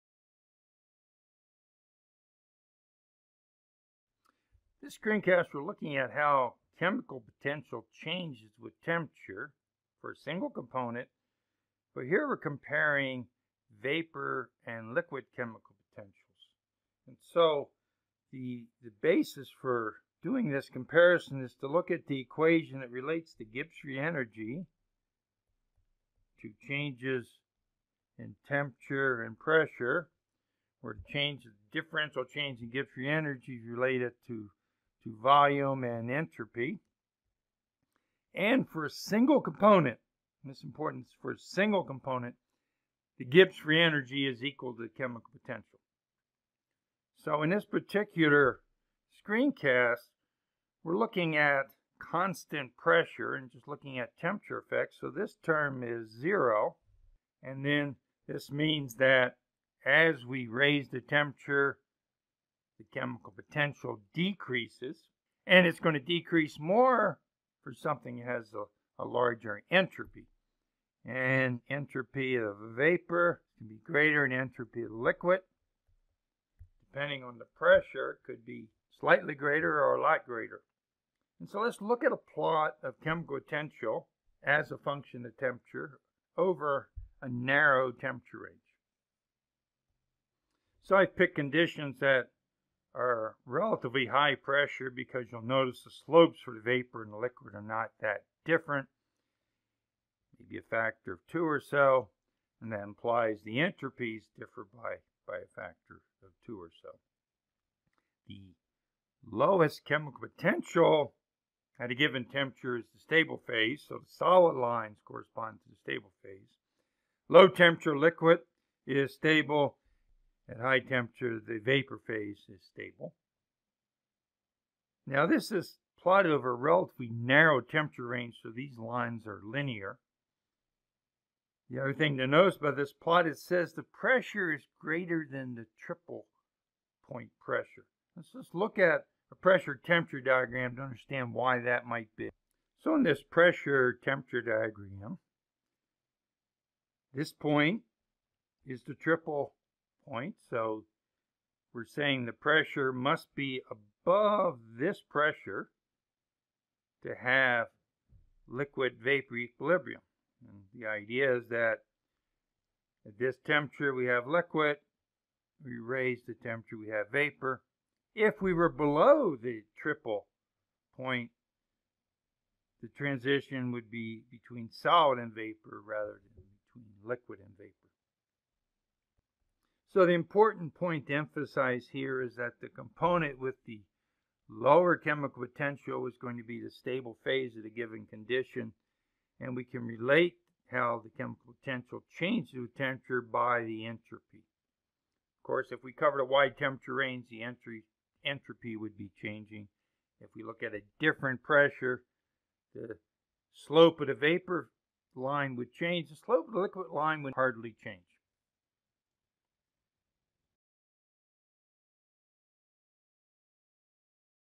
Chemical Potential: Temperature Dependence for Single-Component Part 2
Organized by textbook Shows how the chemical potentials of a vapor and a liquid change at constant pressure as temperature increases over a narrow temperature range for a single component . An interactive simulation is available that demonstrates this behavior Made by faculty at the University of Colorado Boulder, Department of Chemical & Biological Engineering.